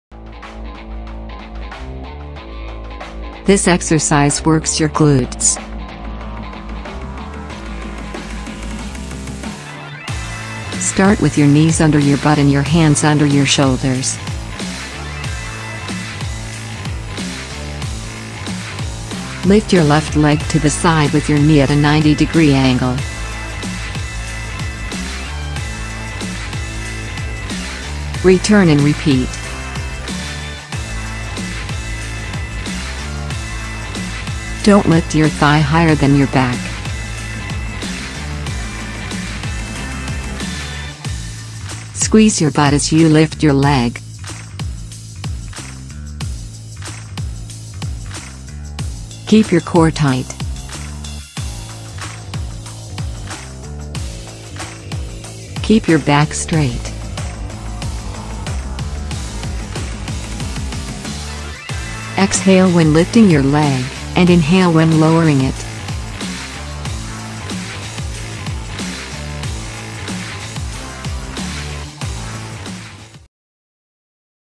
How to Do: FIRE HYDRANT
This exercise works your glutes. Start with your knees under your butt and your hands under your shoulders. Lift your left leg to the side with your knee at a 90 degree angle. Return and repeat.